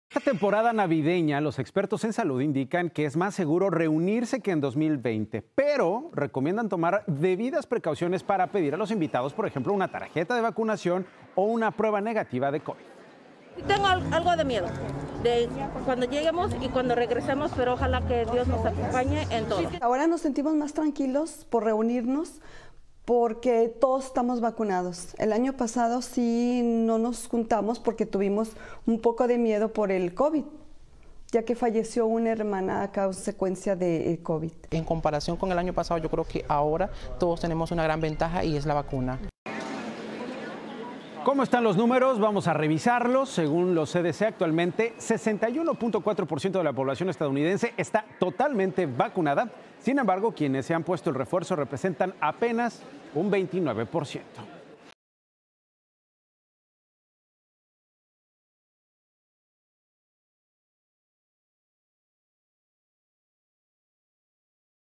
COVID-19: Alertan sobre riesgos en fiestas de fin de año | Noticias Telemundo
Video oficial de Noticias Telemundo. Aunque las autoridades aseguran que este año es más seguro reunirse que en 2020, piden que se tomen medidas para evitar la propagación de la variante ómicron. Según los CDC, 61% de la población de EE. UU. se ha vacunado, pero solo un 29% tiene el refuerzo.